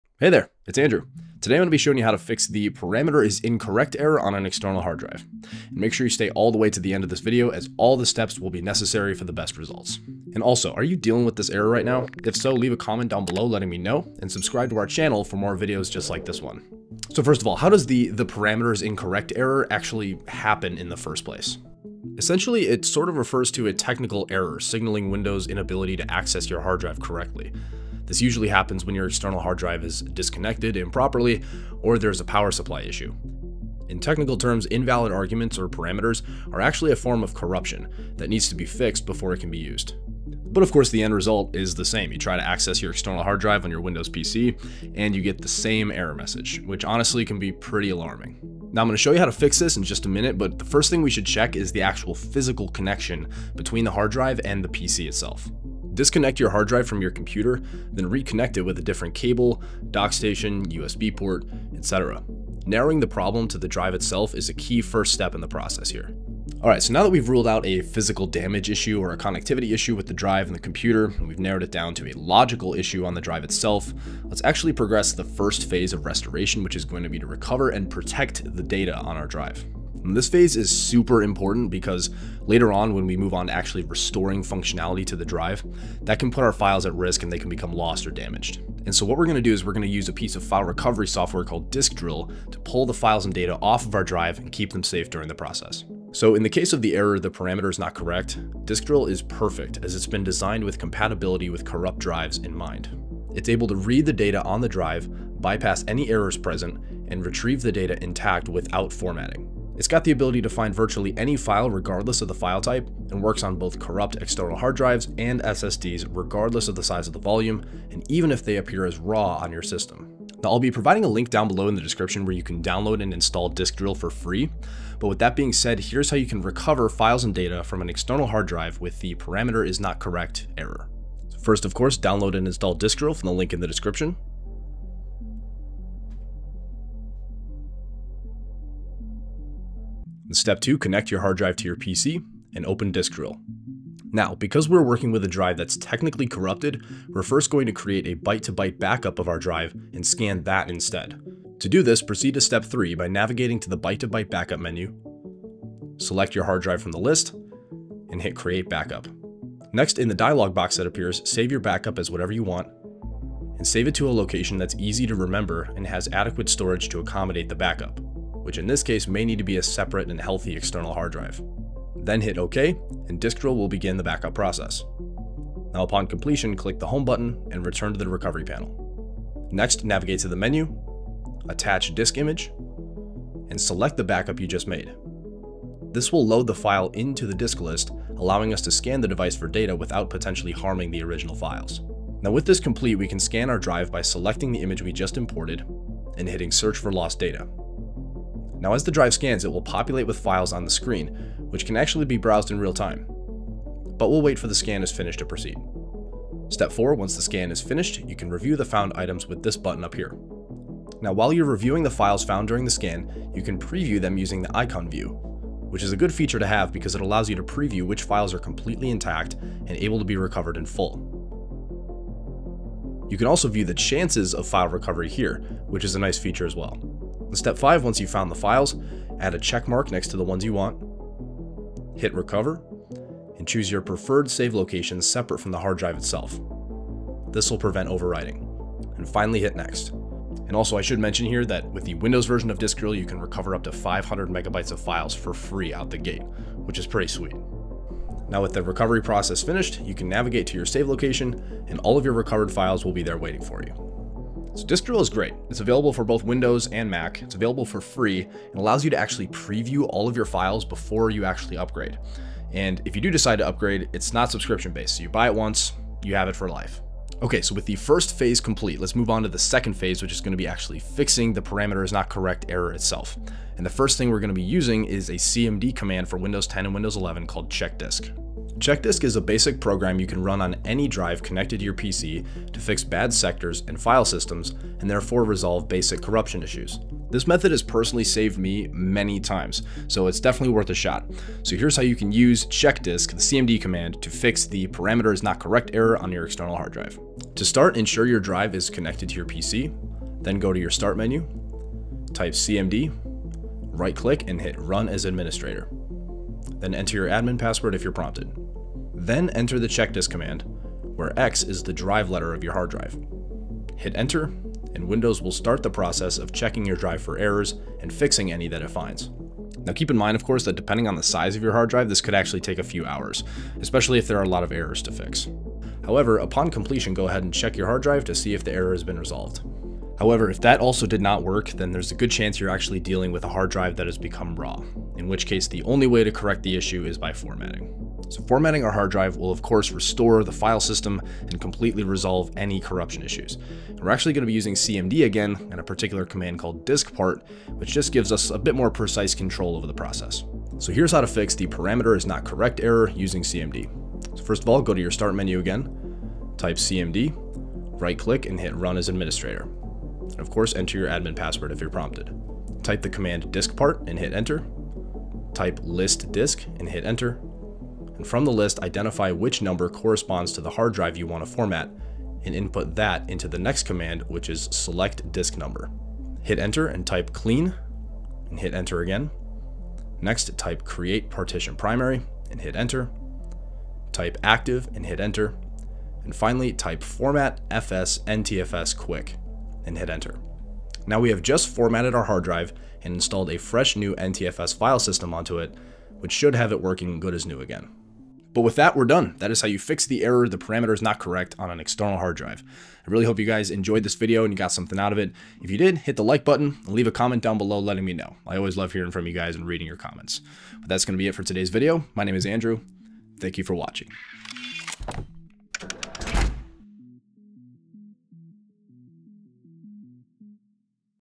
Fix “The Parameter is Incorrect” Error on External Hard Drive
Are you getting the “Parameter Is Incorrect” error when trying to access your external hard drive? In this video, I’ll show you step-by-step how to fix the issue—plus protect your data along the way. ✔️ Hard drive recovery software

Chapters:
00:00 Intro
00:17 What does 'the parameter is incorrect' mean?
00:49 Step 1: Check the physical connection
01:14 Step 2: Recover data from the drive
04:27 Step 3: Fix the error with CHKDSK
05:30 Step 4: Fix error with formatting
06:38 Wrap up

*What Causes the “Parameter Is Incorrect” Error?*
This error typically appears when:
* An external hard drive is disconnected improperly
* There is a power supply issue
* Invalid arguments/parameters (corruption) occur
* Windows can’t access the drive properly.

Below we will give you the correct sequence of steps to successfully extract data from the drive with the parameter is incorrect error and fix the error itself.

🔌 *Step 1: Check physical connections*
* Unplug your external hard drive from the PC.
* Use a different USB cable, dock station, or USB port.
If the drive remains undetected or problematic after changing cables and ports, the issue could be physical. Otherwise, proceed to the logical fixes.

💾 *Step 2: Recover files from the drive with 'the parameter is incorrect' error*
Before trying to fix the error, back up your data—repairs can sometimes cause file damage or loss. We recommend Disk Drill because it can bypass “parameter is incorrect” errors and recover files from corrupted or RAW drives. Thanks to its intuitive interface and thorough scanning methods, Disk Drill can locate and restore virtually any type of file—photos, documents, videos, and more—while providing valuable features like real-time file previews and byte-to-byte backups.
Also, on Windows, you can recover up to 500MB of data for free with Disk Drill.

✅ To recover data from the external hard drive with the 'parameter is incorrect' error:
1.
2. Launch Disk Drill and go to Byte-to-Byte Backup.
3. Select your corrupted drive, choose a backup destination, and click Create Backup.
4. Go back to Disk Drill’s main window, click Attach disk image, and select the backup file you just created.
5. Select the loaded image, click Search for lost data, and wait for the scan to complete.
6. Once scanning is finished, click Review found items. Preview files and check their recovery chances.
7. Select the files you want to recover, click Recover, and choose a safe save location (not the original drive).

With your data safe, it’s time to repair your drive.

🛠️ *Method 1: Use CHKDSK (Windows 10 & 11)*
To fix the parameter is incorrect on an external hard drive using CMD:
1. Go to Start, type cmd, right-click Command Prompt, and select Run as administrator.
2. Type chkdsk X: /f (Replace X with your drive letter.)
3. Press Enter to start scanning and repairing.
4. The scan may take a while if the drive is large. Check if the error is resolved.

🔨 *Method 2: Format Using DiskPart (For RAW Drives)*
If your drive is RAW or still not fixed, formatting is often the only solution.
To fix the external hard drive parameter is incorrect error by formatting:
1. Open Command Prompt as Administrator
2. Access DiskPart
3. Then write the following sequence:
* diskpart
* list disk
* select disk #
* clean
* create partition primary
* active
* format fs=ntfs quick
(Replace # with the number of your external drive.)
4. After formatting, your drive should work like new.